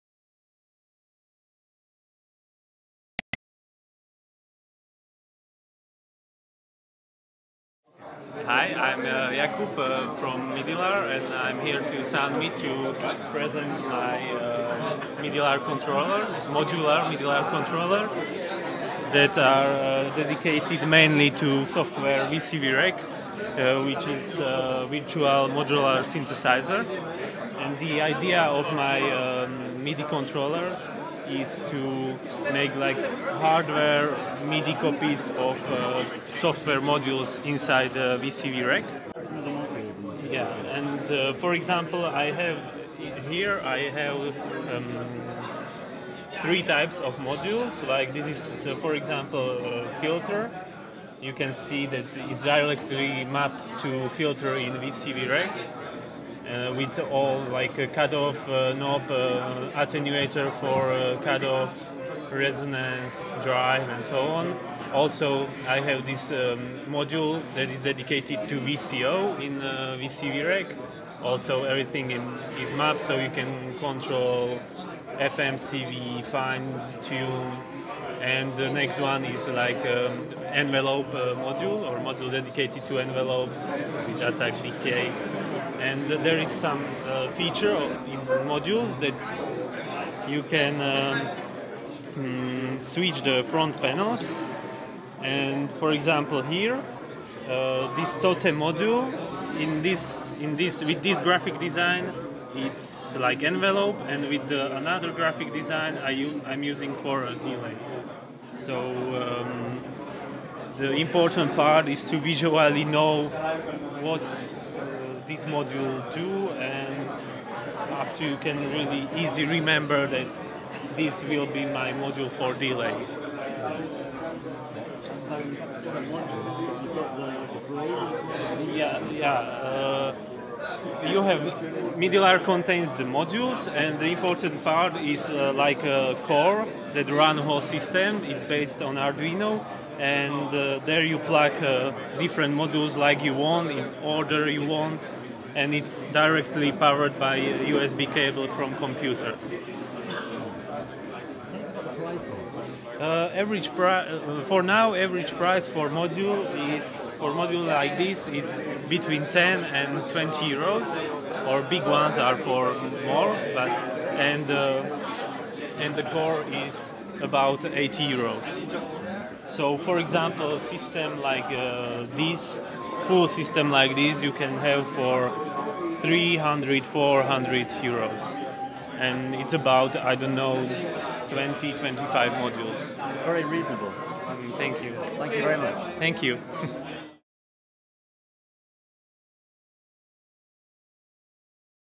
Soundmit 2019: MIDILAR Modular MIDI Controller
Imagine being able to control VCV rack with hardware that is super affordable and will essentially give you a eurorack-style setup for a fraction of the cost... this is MIDILAR.